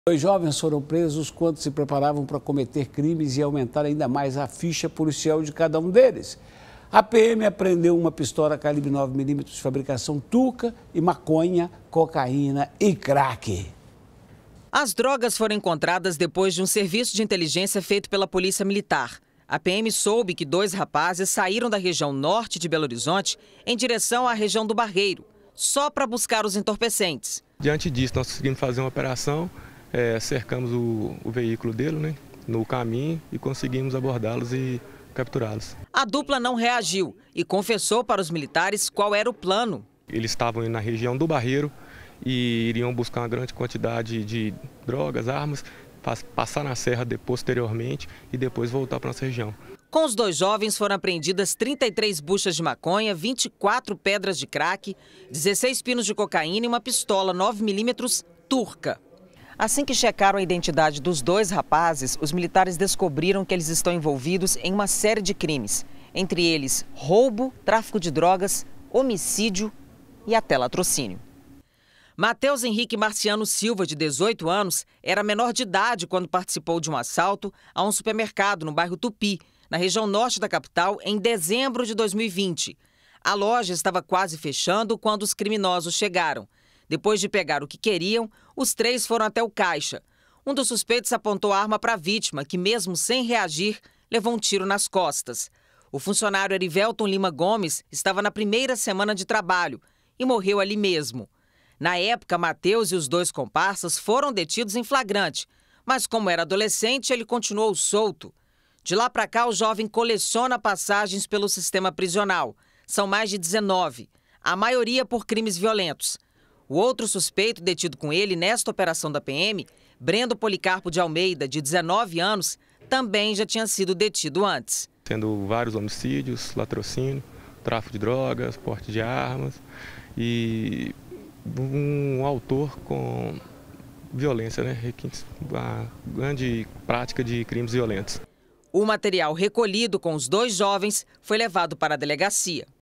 Jovens envolvidos com o tráfico são presos em Belo Horizonte
Dois jovens foram presos quando se preparavam para cometer crimes na noite desta segunda-feira (25). A polícia militar apreendeu uma pistola calibre 9 milímetros de fabricação turca, além de maconha, cocaína e crack com os presos.